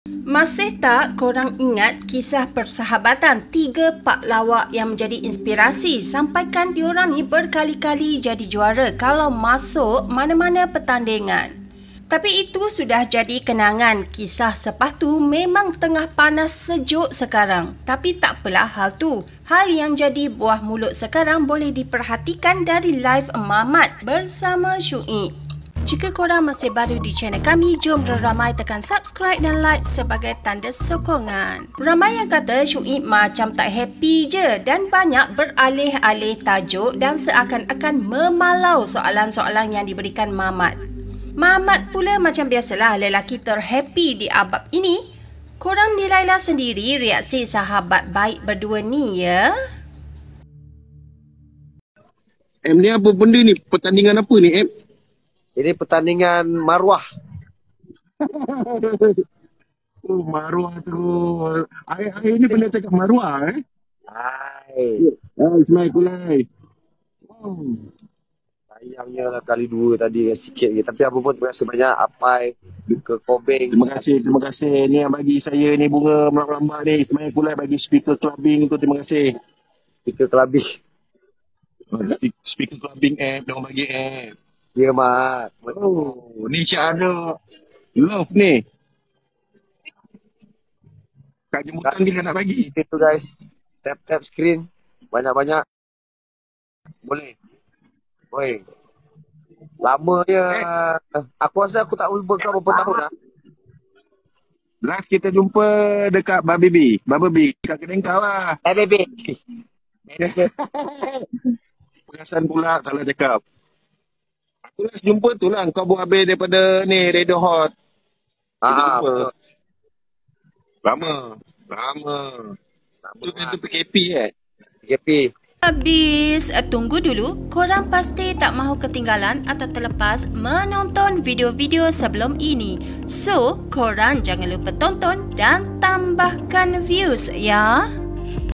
SEDIH BETUL DENGAR SUARA MAMAT DALAM LIVE NI! RAMAI PULAK PERASAN ADA YANG TAK KENA DENGAN SHUIB?
😊 Do alert me anything with comments if you have any enquiries

😊 Sumber Berita 😃 Google 🌍
😊 Sumber Visual 😃 TT : ShuibPalauMamat | Google File 🎞
😊 Sumber Muzik 🎧 YT Library 🎵
😊 Video & Voice Over 😃